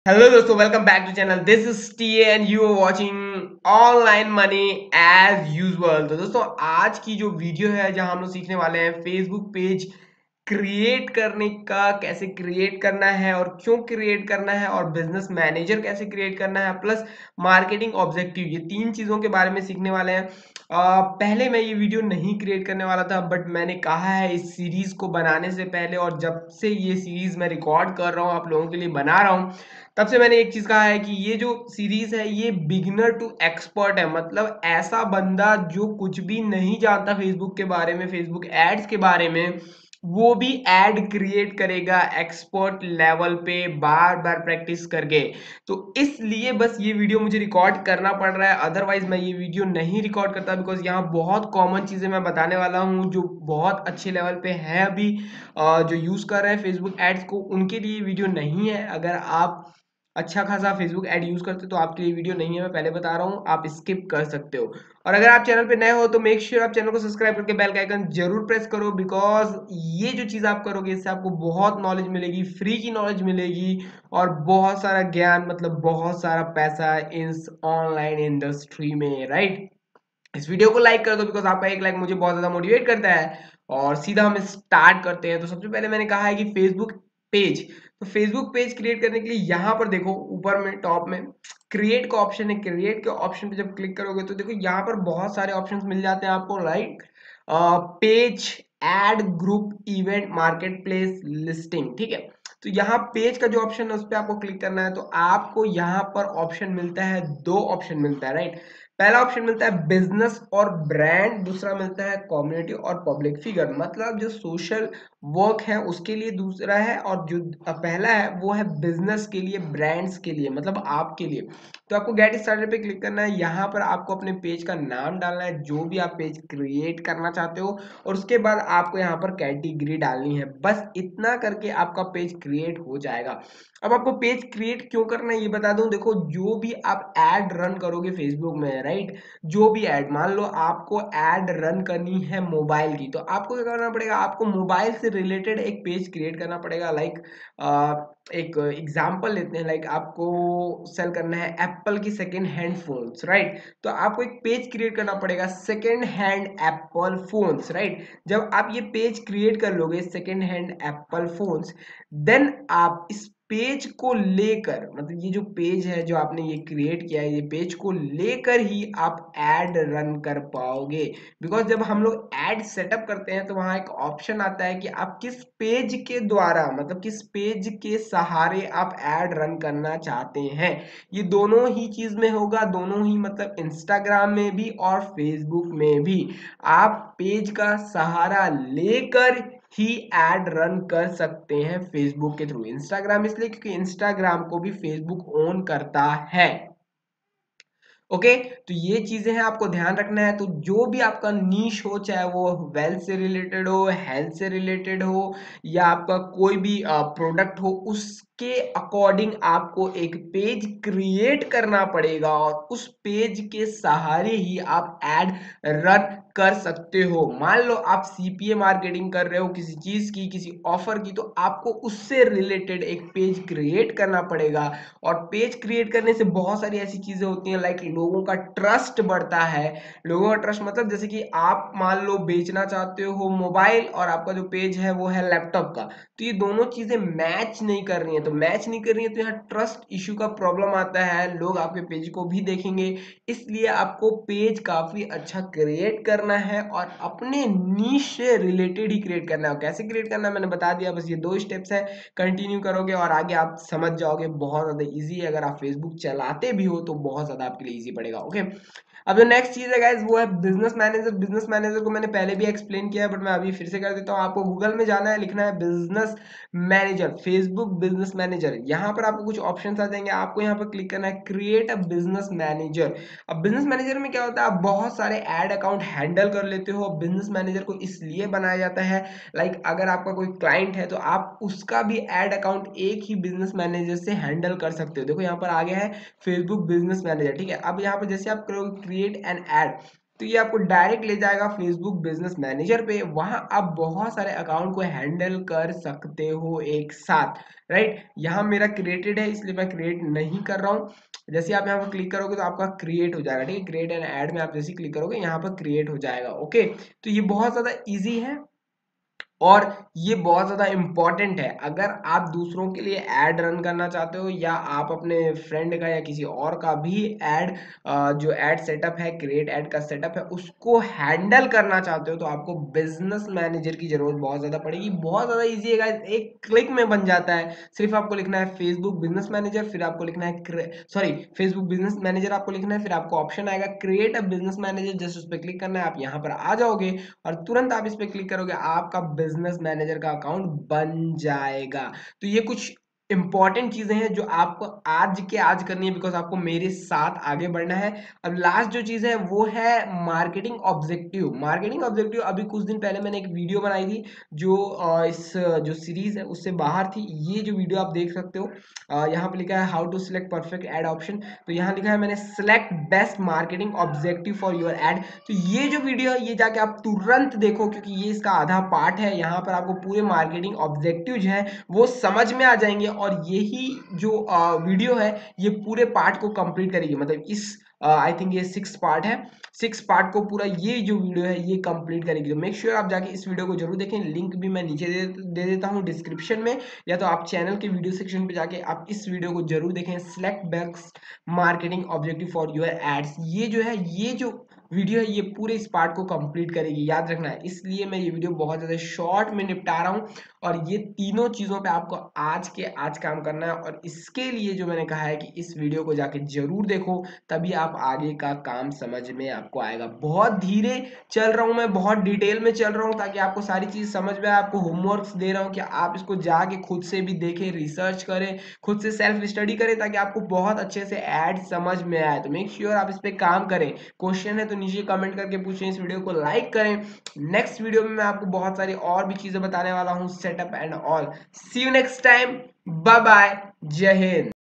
Create business manager and Facebook page | Facebook marketing objective | fb ads mastery part- 6
Create business manager and Facebook page | Facebook marketing objective | fb ads mastery part- 6 [ Hindi/Urdu ]

SAY ME HI! HERE - On Instagram- @touhidahmadofficial

KEEP CONNECT
BE HAPPY.